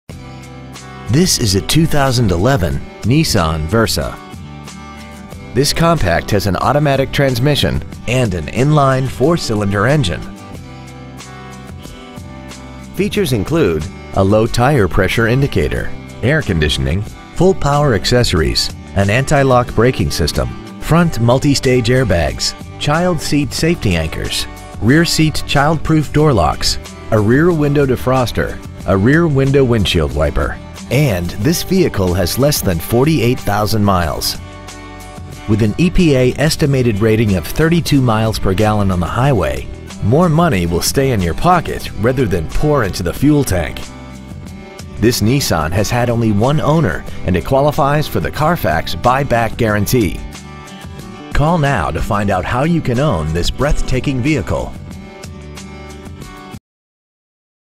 2011 Versa Mesa AZ 85207
We are proud to present this 2011       Versa .

 We have been honored to serve the Mesa AZ area , we promise that your experience at our dealership will exceed your expectations !

 Year : 2011
 Make :
 Model : Versa
 Engine : 1.8L DOHC 16-valve CVTCS I4 engine
 Trans . : Automatic
 Exterior : Other
 Miles : 47,493
 Interior : Other